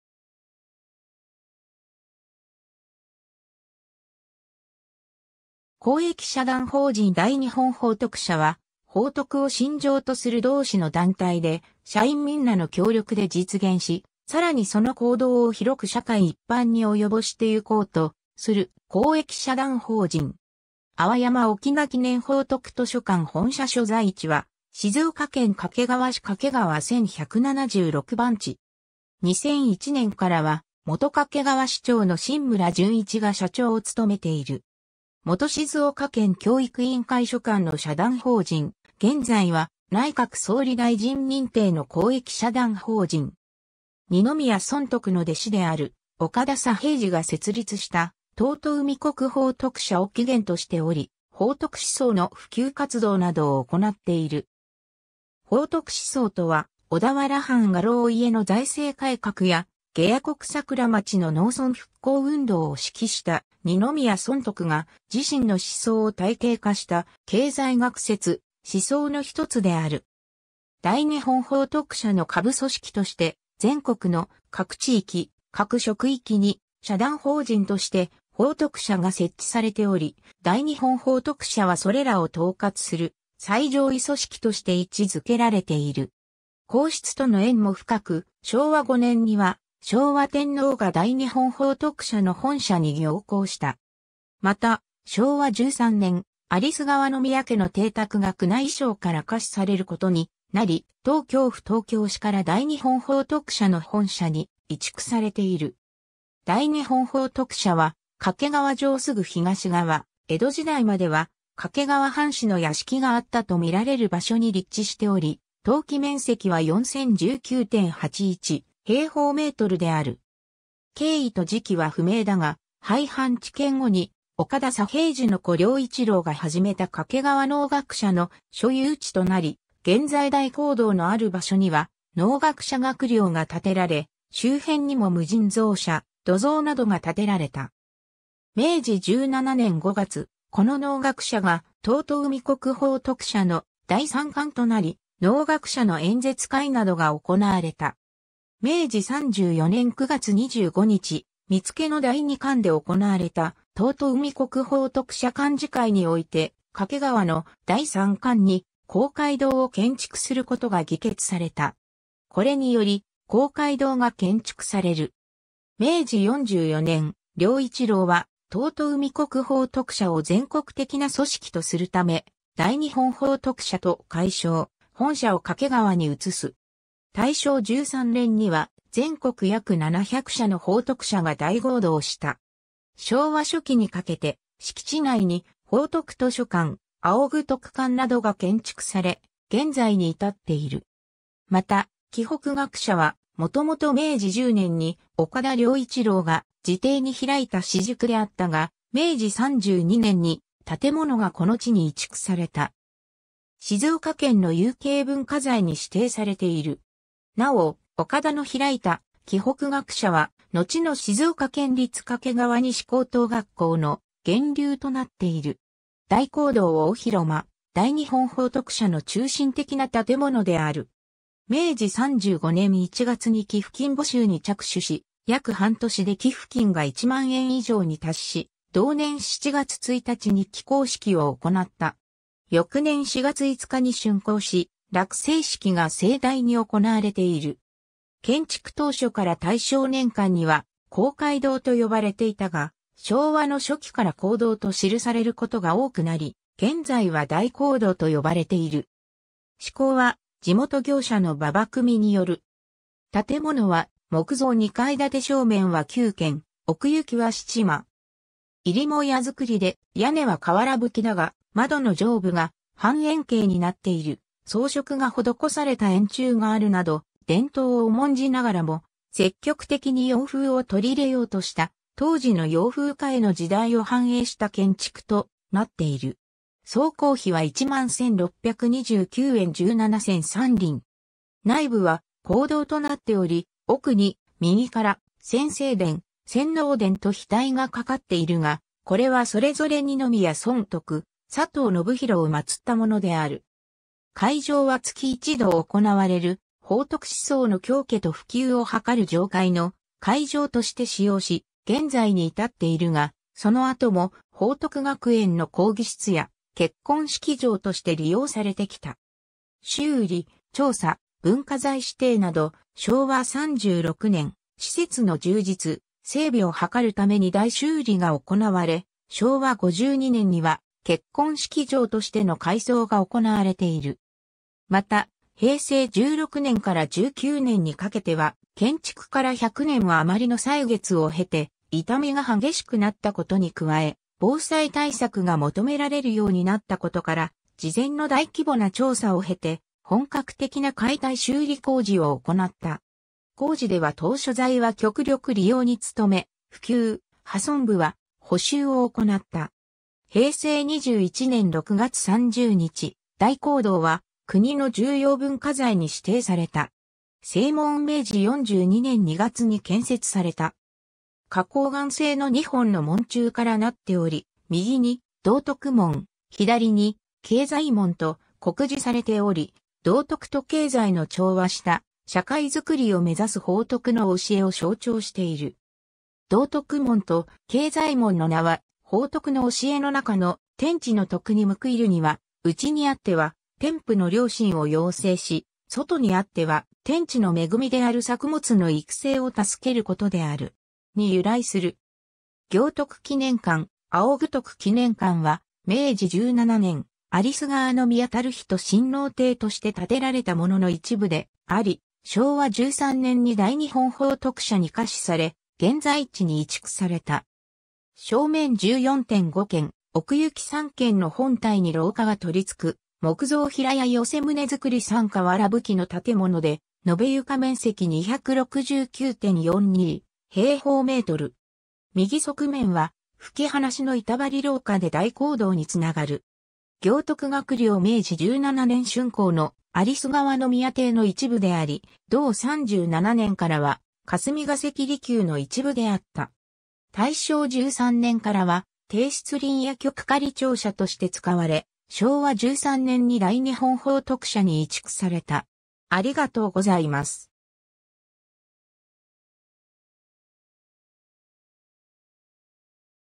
大日本報徳社
公益社団法人_(内閣総理大臣認定)
重要文化財_(建造物)
公益社団法人大日本報徳社（だいにっぽんほうとくしゃ）は、報徳を信条とする同志の団体で、社員みんなの協力で実現し、さらにその行動を広く社会一般に及ぼしてゆこうとする公益社団法人。
 淡山翁記念報徳図書館（静岡県指定有形文化財） 本社所在地は静岡県掛川市掛川1176番地。
2001年からは元掛川市長の榛村純一が社長を務めている。
元静岡県教育委員会所管の社団法人、現在は内閣総理大臣認定の公益社団法人。
二宮尊徳の弟子である岡田佐平治が設立した「遠江国報徳社」を起源としており、「報徳思想」の普及活動などを行っている。
 報徳思想とは、小田原藩家老家の財政改革や下野国桜町の農村復興運動を指揮した二宮尊徳が、自身の思想を体系化した経済学説・思想の一つである。
大日本報徳社の下部組織として全国の各地域、各職域に社団法人として「報徳社」が設置されており、大日本報徳社はそれらを統括する最上位組織として位置づけられている。
 皇室との縁も深く、昭和5年には昭和天皇が大日本報徳社の本社に行幸した。
また、昭和13年、有栖川宮家の邸宅が宮内省から下賜されることになり、東京府東京市から大日本報徳社の本社に移築されている。
 大日本報徳社は、掛川城すぐ東側、江戸時代までは掛川藩士の屋敷があったと見られる場所に立地しており、登記面積は4,019.81m2である。
経緯と時期は不明だが、廃藩置県後に岡田佐平治の子良一郎が始めた掛川農学社の所有地となり、現在大講堂のある場所には農学社学寮が建てられ、周辺にも無尽蔵舎、土蔵などが建てられた。
明治17年（1885年）5月、この農学社が遠江国報徳社の第3館（第1館は浜松、第2館は見付（現在の磐田市中心部））となり、農学社の演説会などが行われた。
明治34年（1901年）9月25日、見付の第2館で行われた遠江国報徳社幹事会において、掛川の第3館に公会堂を建築することが議決された。
これにより公会堂が建築される。
明治44年（1924年）、良一郎は遠江国報徳社を全国的な組織とするため「大日本報徳社」と改称、本社を掛川に移す。
大正13年（1924年）には全国約700社の報徳社が大合同した。
昭和初期にかけて敷地内に報徳図書館、仰徳館などが建築され、現在に至っている。
 また、冀北学舎は、もともと明治10年に岡田良一郎が自邸に開いた私塾であったが、明治32年に建物がこの地に移築された。
静岡県の有形文化財に指定されている。
なお、岡田の開いた冀北学舎は、のちの静岡県立掛川西高等学校の源流となっている。
 大講堂大広間 大日本報徳社の中心的な建物である。
明治35年1月に寄付金募集に着手し、約半年で寄付金が1万円以上に達し、同年7月1日に起工式を行った。
翌年4月5日に竣工し、落成式が盛大に行われている。
建築当初から大正年間には公会堂と呼ばれていたが、昭和の初期から講堂と記されることが多くなり、現在は大講堂と呼ばれている。
 施工は地元業者の馬場組による。
建物は木造2階建て正面は9間（20m）、奥行きは7間（15.9m）。
入母屋作りで屋根は瓦葺きだが、窓の上部が半円形になっている、装飾が施された円柱があるなど、伝統を重んじながらも積極的に洋風を取り入れようとした、当時の洋風化への時代を反映した建築となっている。
総工費は11,629円17銭3厘。
内部は講堂となっており、奥に右から「先聖殿」「先農殿」と額がかかっているが、これはそれぞれ二宮尊徳、佐藤信淵を祀ったものである。
会場は月一度行われる報徳思想の教化と普及を図る常会の会場として使用し、現在に至っているが、その後も報徳学園の講義室や結婚式場として利用されてきた。
 修理・調査・文化財指定など 昭和36年（1961年）、施設の充実・整備を図るために大修理が行われ、昭和52年（1977年）には結婚式場としての改装が行われている。
また、平成16年（2004年）から19年にかけては、建築から100年あまりの歳月を経て痛みが激しくなったことに加え、防災対策が求められるようになったことから、事前の大規模な調査を経て、本格的な解体修理工事を行った。
工事では当初材は極力利用に努め、腐朽・破損部は補修を行った。
 平成21年（2009年）6月30日、大講堂は国の重要文化財に指定された。
 正門（道徳門と経済門） 明治4...